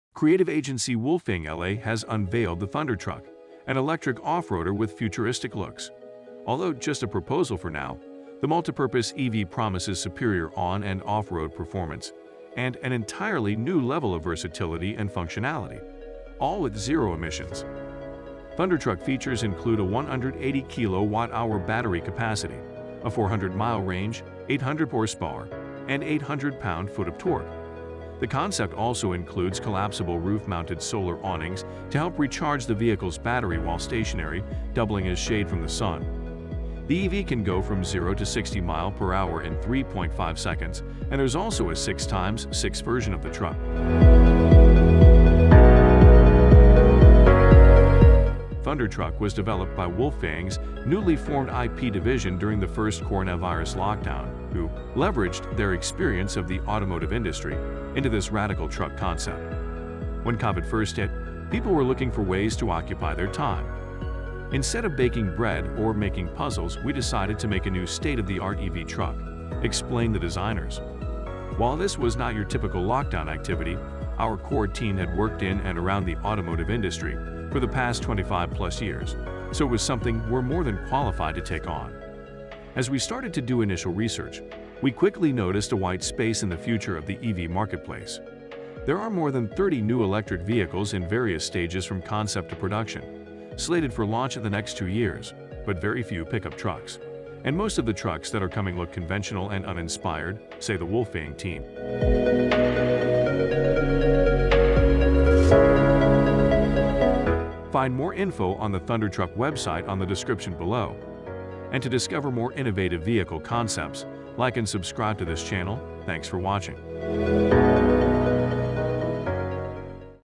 Meet Thundertruck : Electric Offroader with 'BAT WING' solar awnings
meet thundertruck: the electric offroader with 'bat wing' solar awnings,
thundertruck was developed by wolfgang’s newly formed IP division during the first coronavirus lockdown, who leveraged their experience of the automotive industry into this radical truck concept.